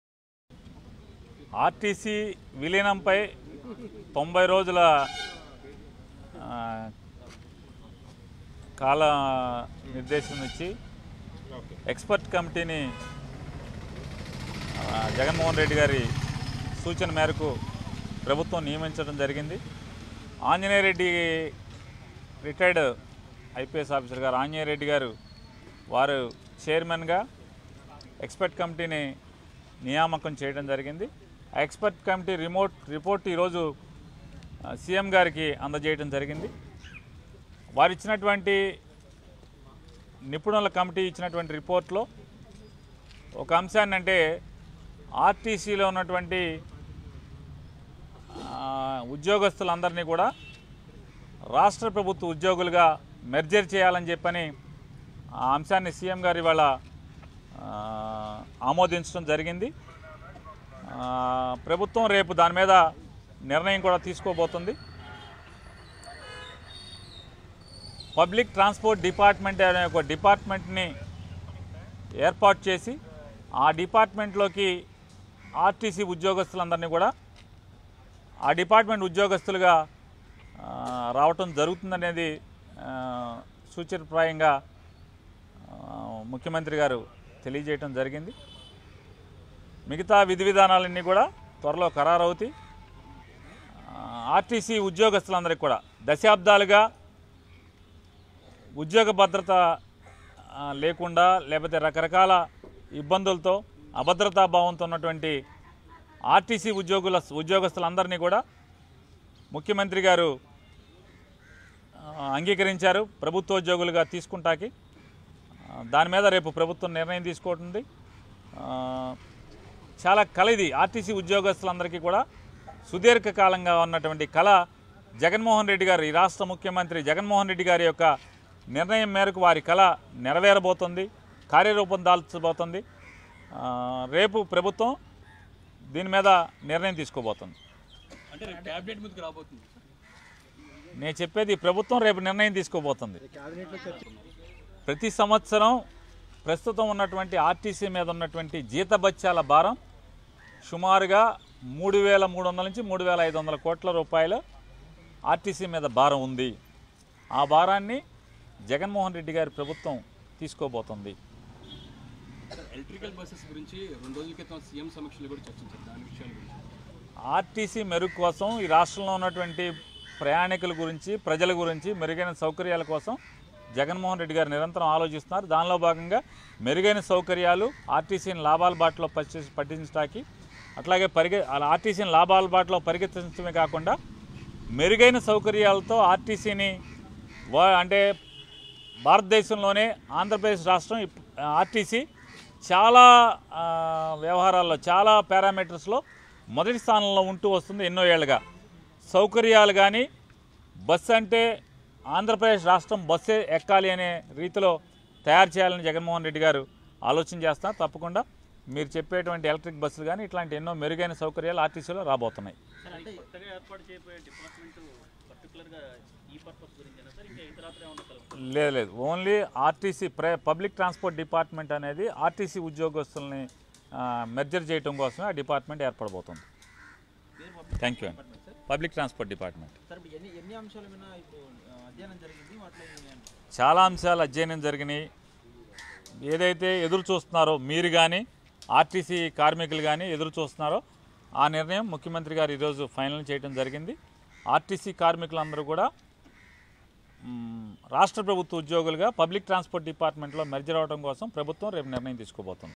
AP Transport Minister Perni Nani on dilution of APS RTC in government
AP Transport Minister Perni Nani on dilution of APS RTC in government - 3rd Sep 2019